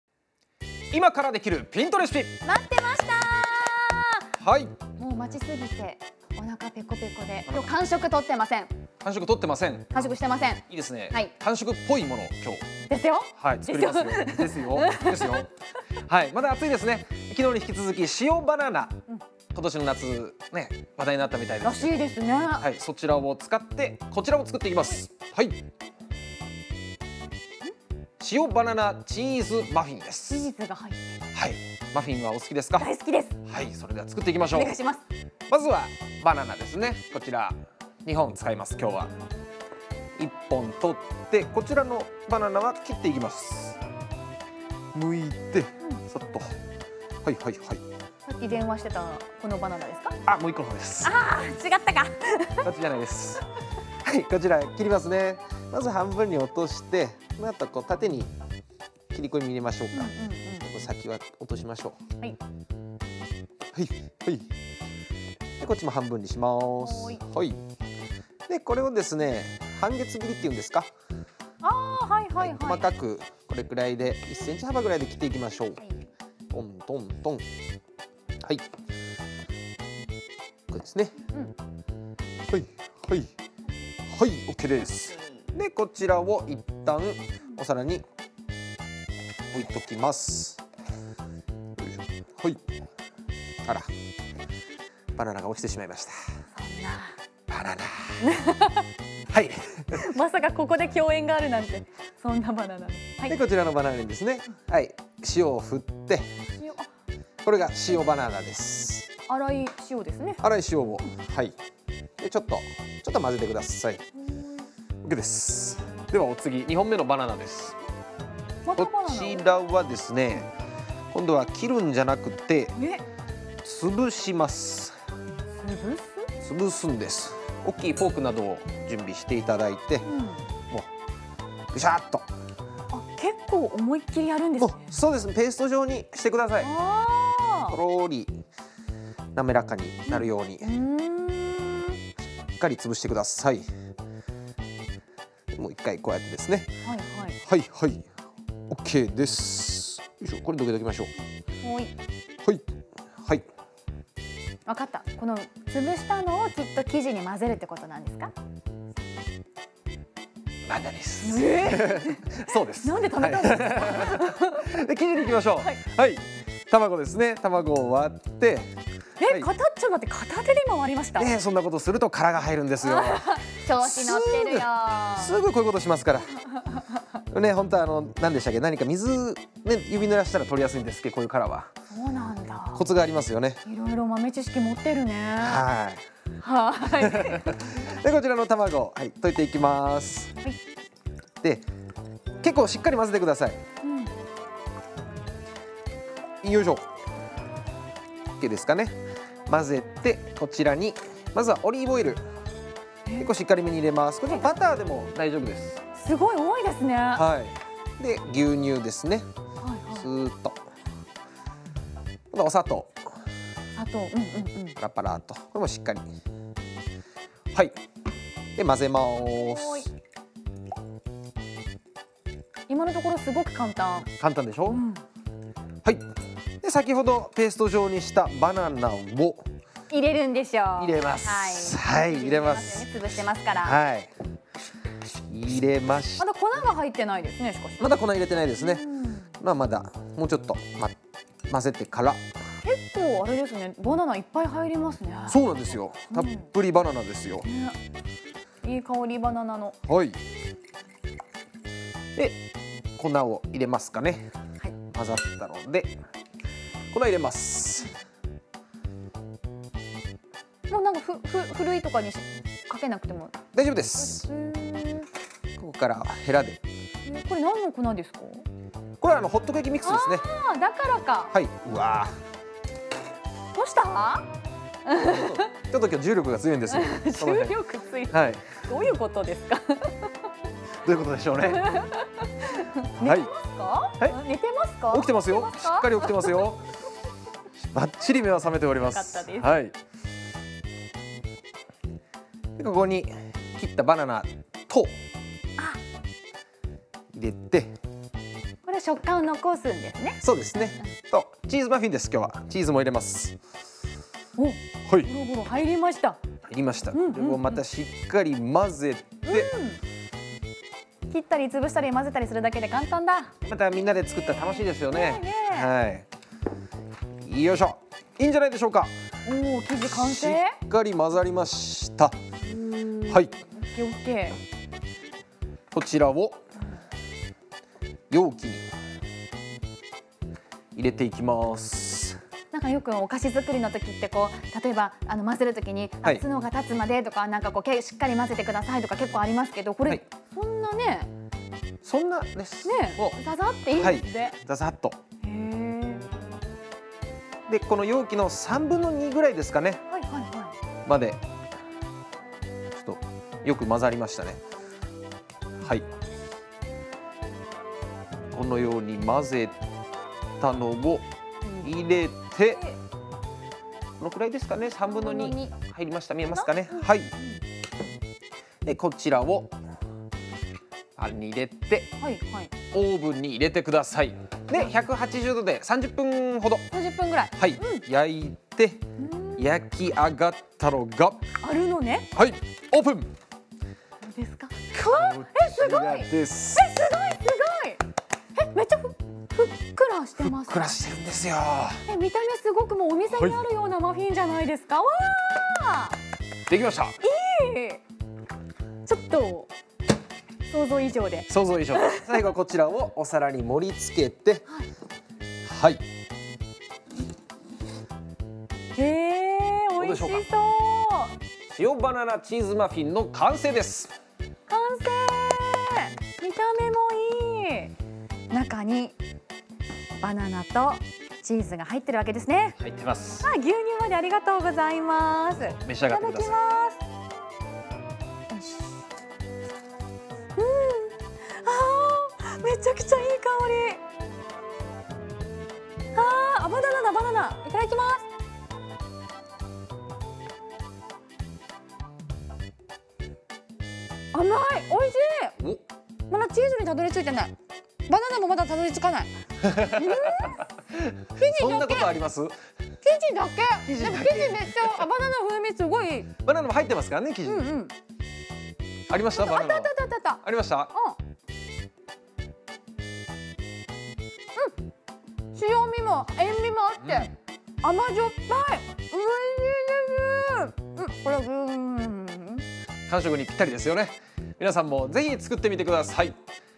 いまからできるPintレシピ「塩バナナチーズマフィン」
【材料】
バナナ：2 本
プロセスチーズ：適量
ホットケーキミックス：１５０ｇ
たまご：1 個
牛乳：大さじ２
オリーブ油：大さじ２
砂糖：大さじ２
塩：適量

【作り方】
1. バナナは、1 本は小さく切って塩をまぶしておく。もう 1 本はフォークなどで潰しておく。
2. ボウルに卵を割って溶き、牛乳、砂糖、オリーブ油、①の潰したバナナを入れて混ぜる。ホットケーキミックスも加えて混ぜる。
3. 残りのバナナとサイコロ状に切ったチーズも入れて混ぜる。
4. マフィン型に流し入れ、180℃のオーブンで 30 分ほど焼く。